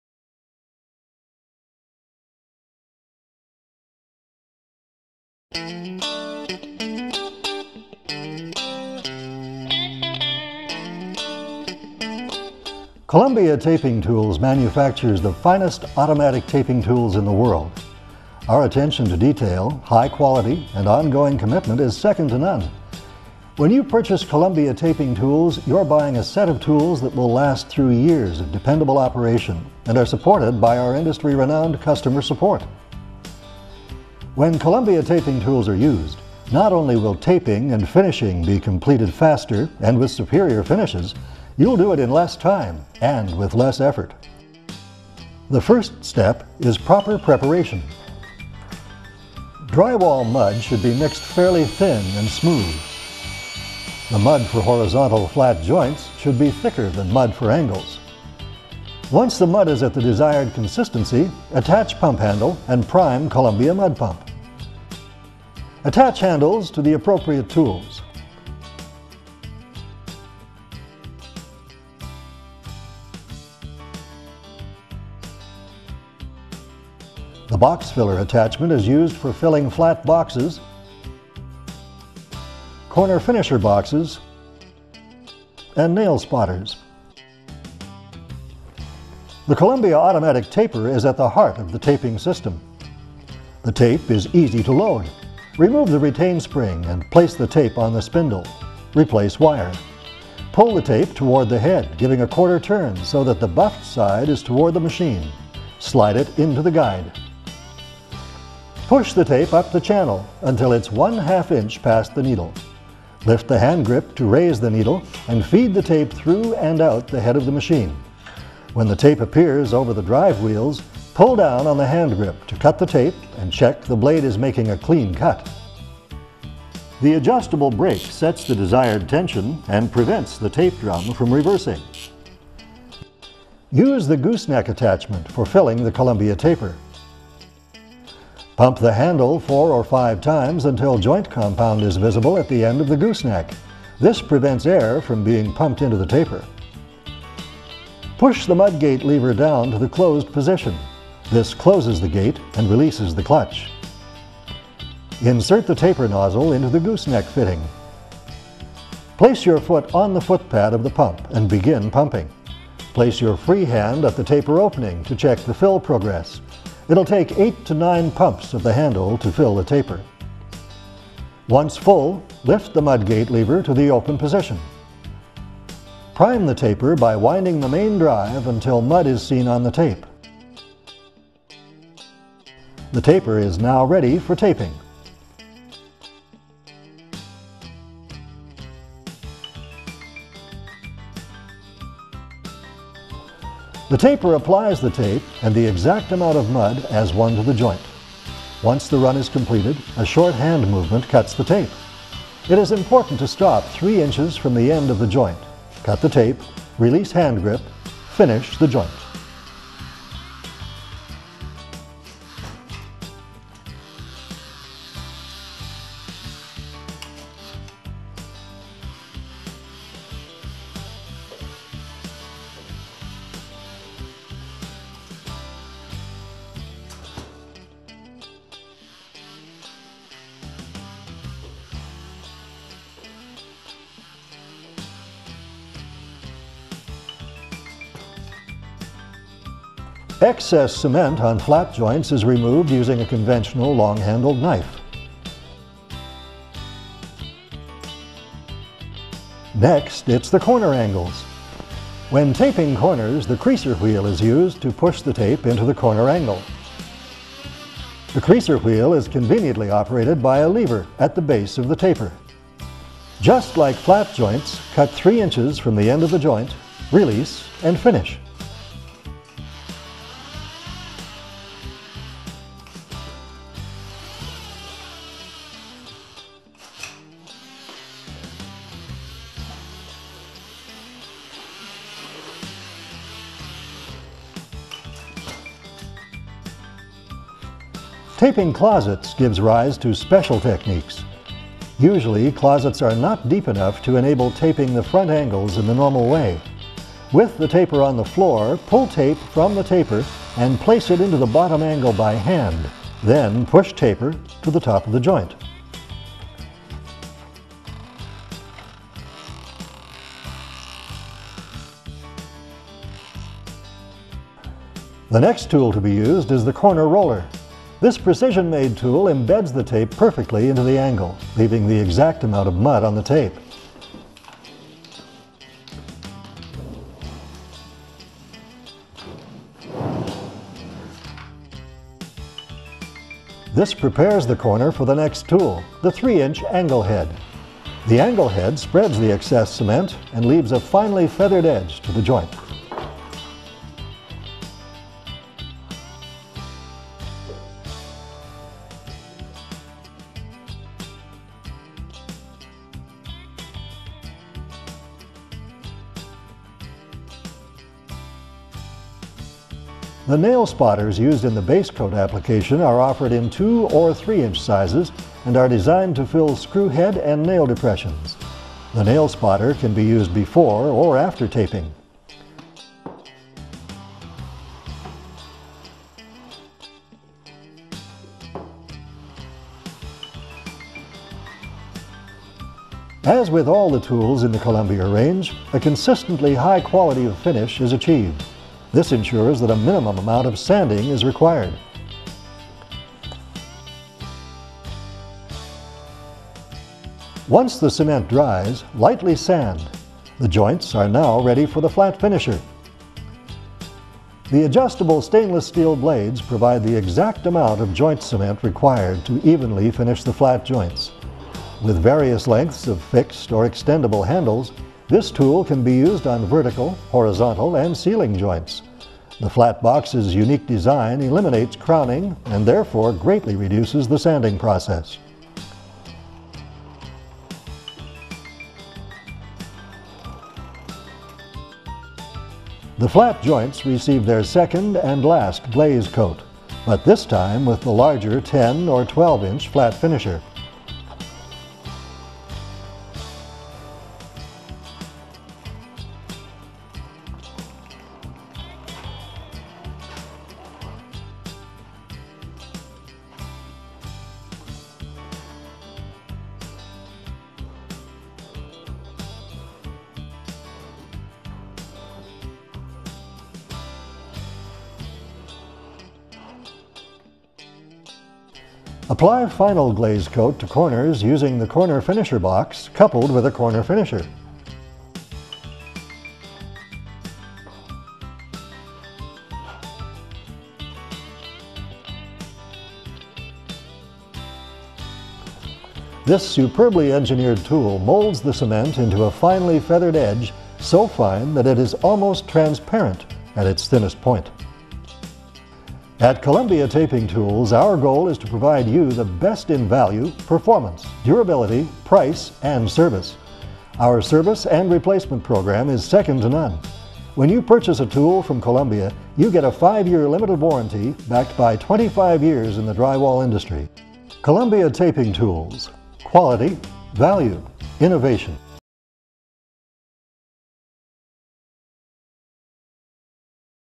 Columbia Taping Tools from Pro Plaster Products
Columbia Taping Tools pride themselves on innovative design, quality manufacturing and over 3 decades of industry experience. The results of Columbias superior engineering, craftsmanship and service are proven daily among the worlds top commercial and residential plastering contractors around the world. Columbia Taping Tools are specifically designed to run at peak performance in the most demanding commercial environments.

Columbia offers a full range of sleek looking automatic taping tools that will ensure you save time on the job, whilst presenting a professional finish every time! This range of automatic taping tools comes with a 5 year warranty, with servicing and parts available from Pro Plaster Products.